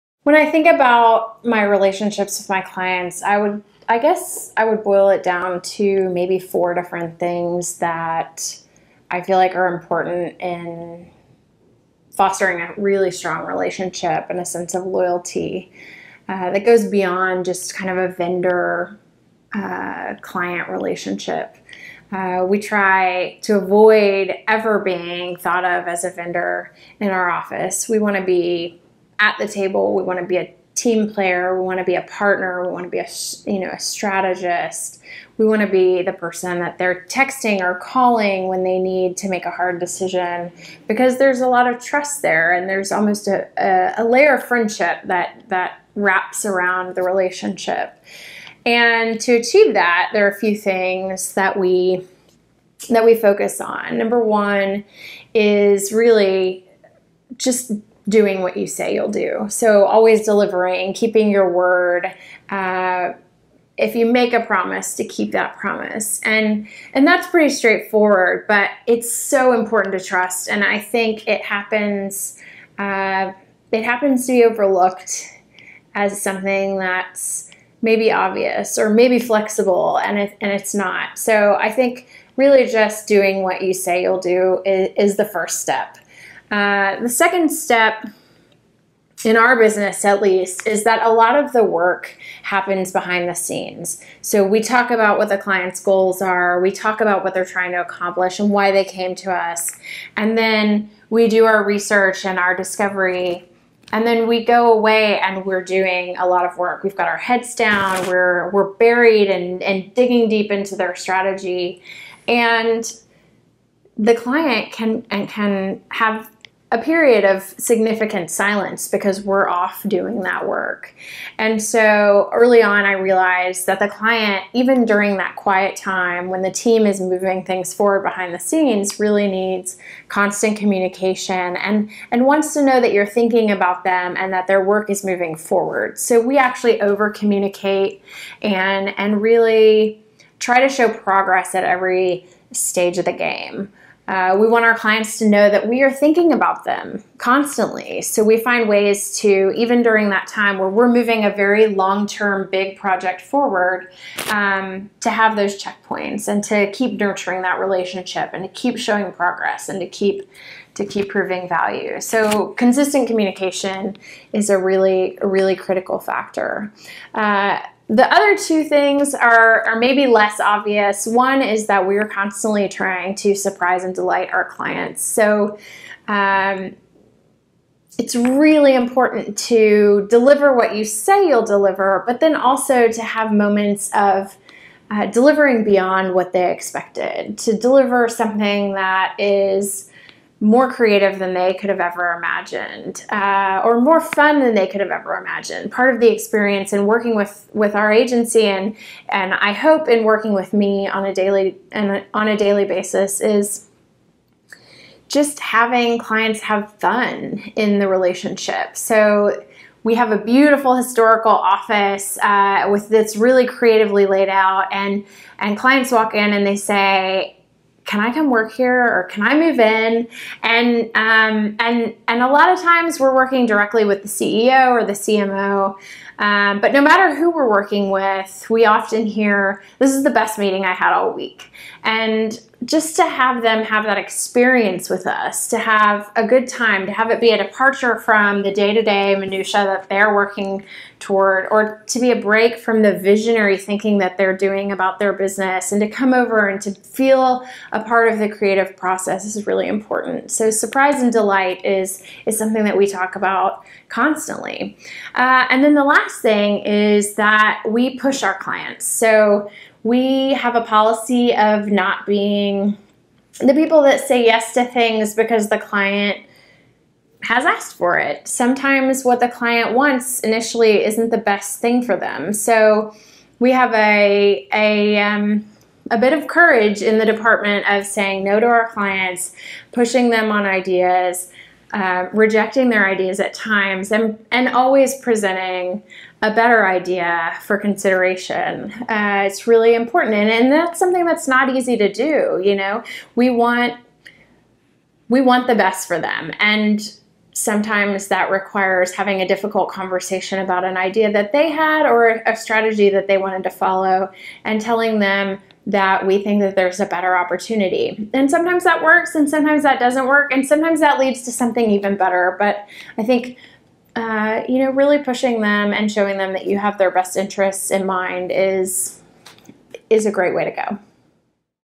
Entrepreneurship: Fostering Loyalty With Clients, Aimee Woodall | WatchHerWorkTV
For more great career advice, visit WatchHerWork.com!